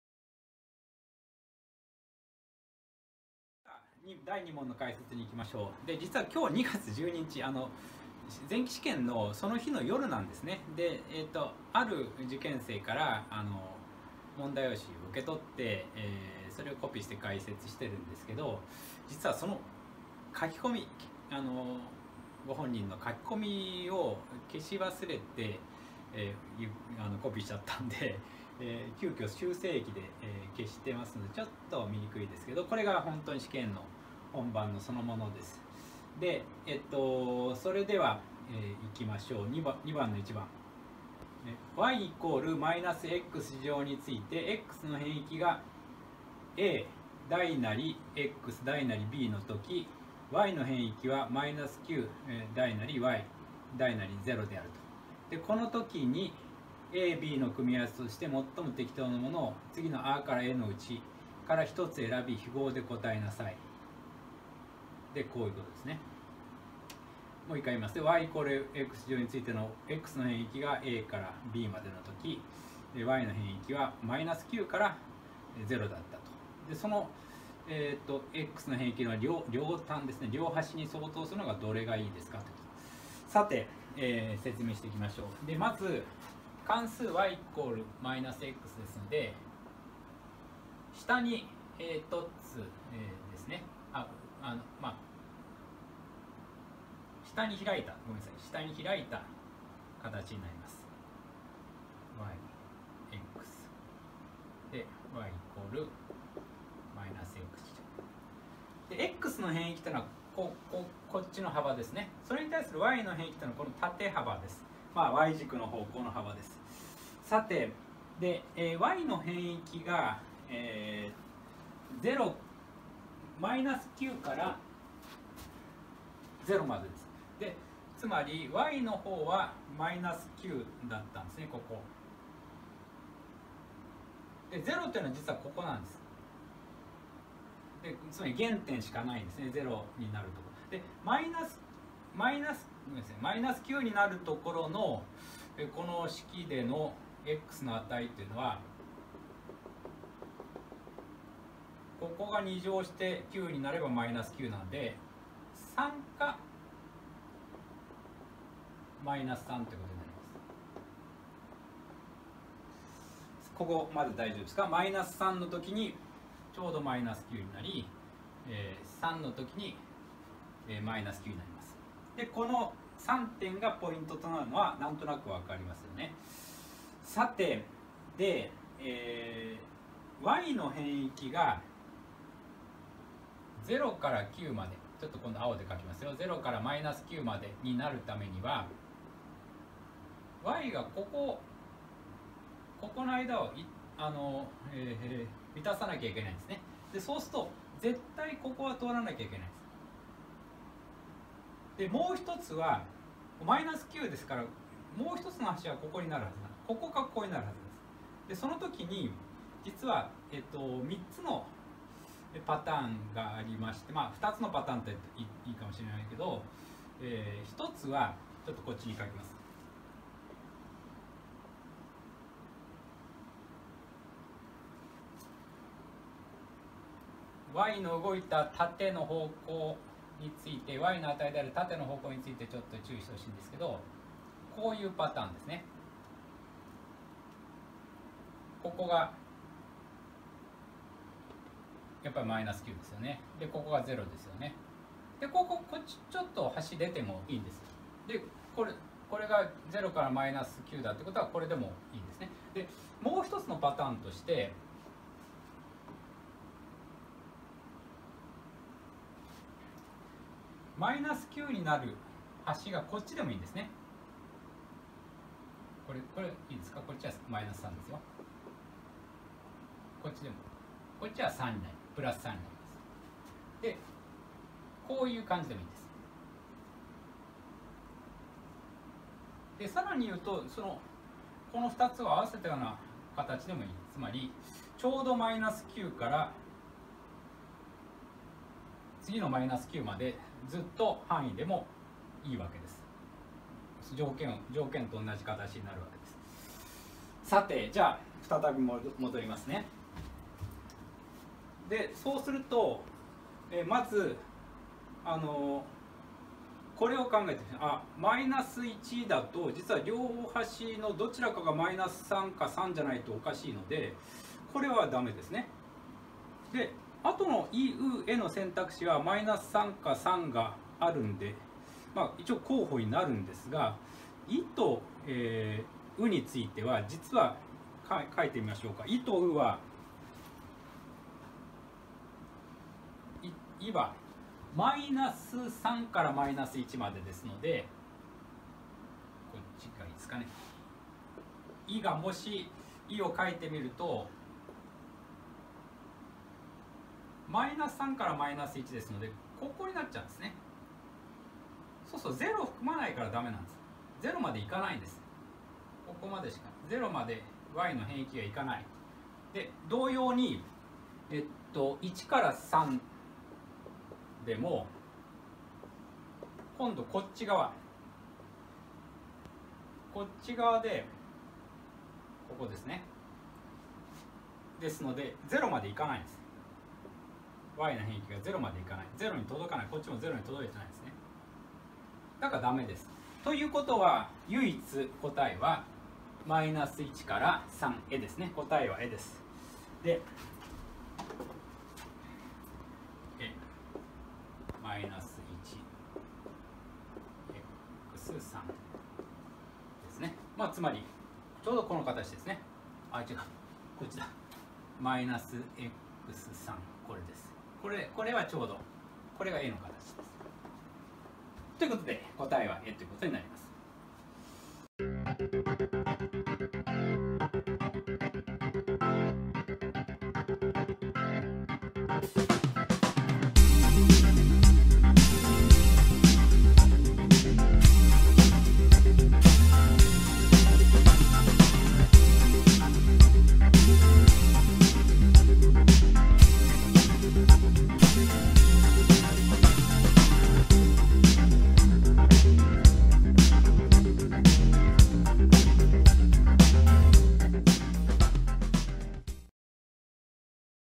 【千葉県】公立高校入試 2020年｜数学｜第2問（1）の解答・実況解説
千葉県の2020年（令和2年）公立高校入試問題から、数学問題の解答をゆっくりとわかりやすく解説！
この動画では数学実況のvol.3として、第2問（1）の解答を解説しています。

▼制作　        朝倉幹晴（船橋市議・予備校講師）
▼動画編集　PINKORO
▼撮影協力　山崎遼太朗（千葉大学2年）
▼撮影　2020年2月12日（映像制作2月13日）